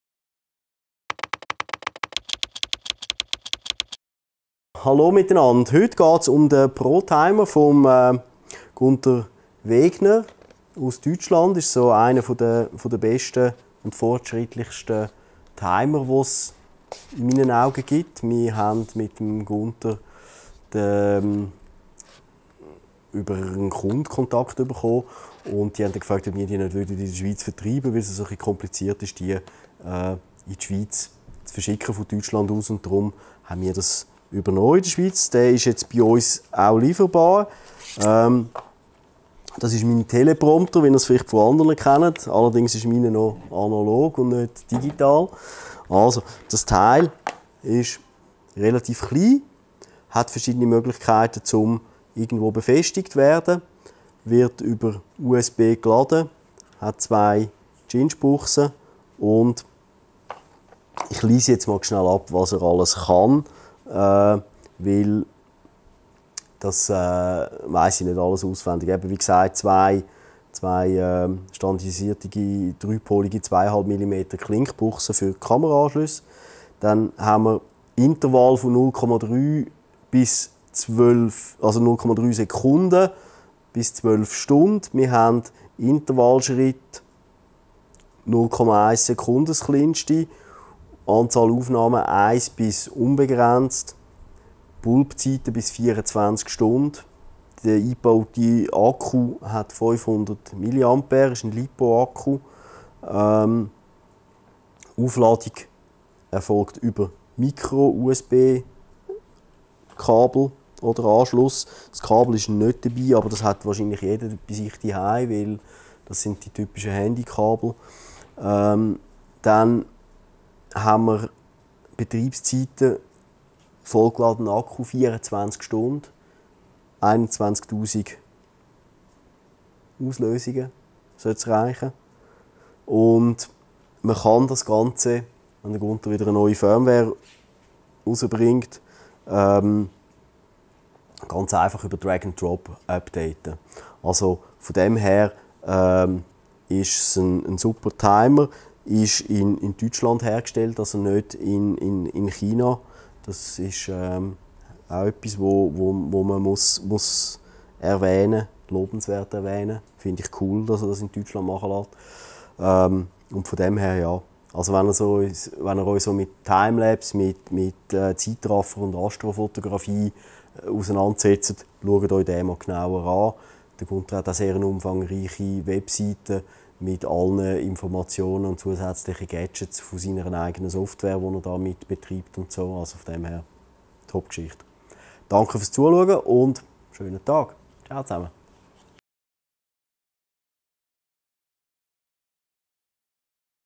Zeitraffer und Astro-Fotografie mit dem LRT Pro Timer - DER beste Intervallauslöser
Der LRTimelapse Pro Timer 2.5 ist einer der fortschrittlichsten und flexibelsten Intervall-Auslöser für Zeitraffer und Astro-Fotografen. Er wurde von Gunther Wegner, einem der Pioniere der Zeitraffer-Fotografie "Made in Germany" entwickelt und speziell auf die Bedürfnisse von anspruchsvollen Zeitraffer- und Astro-Fotografen ausgelegt.

- Sehr performante Auslösung der Kamera, ohne unnötige Verzögerungen durch Autofokus-Signale.
- Kürzeste Intervalle und Schwarzzeiten möglich.
- Ideale Eignung für Aufnahmen des “Heiligen Grals” der Zeitraffer-Fotografie: perfekten Tag-zu-Nacht und Nacht-zu-Tag Aufnahmen. Mehr dazu in einem speziellen Tutorial zu Heiliger-Gral-Aufnahmen von Gunter Wegner